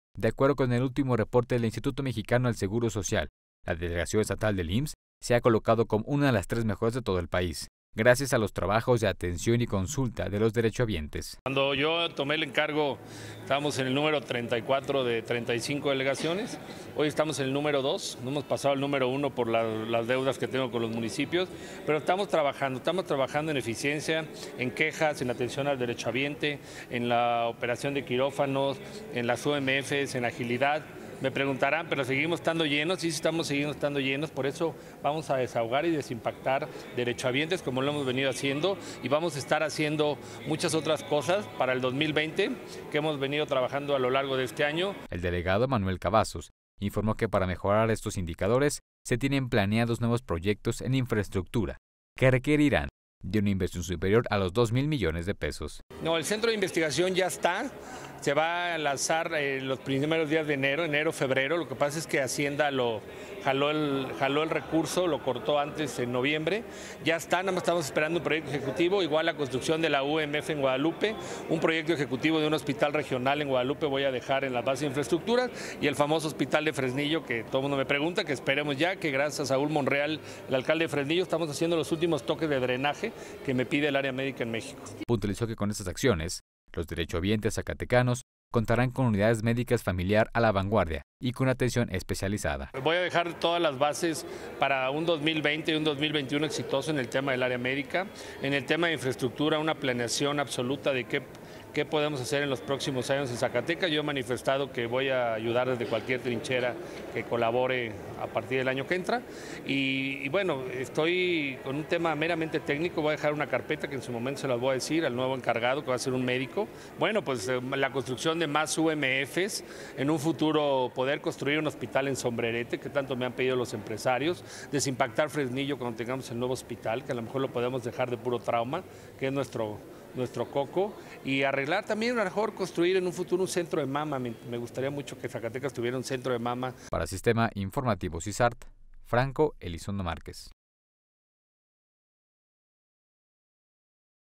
Pretenden innovar IMSS
Para 2020 se tienen planeados nuevos proyectos en infraestructura en  la delegación estatal del  Instituto Mexicano del Seguro Social (IMSS).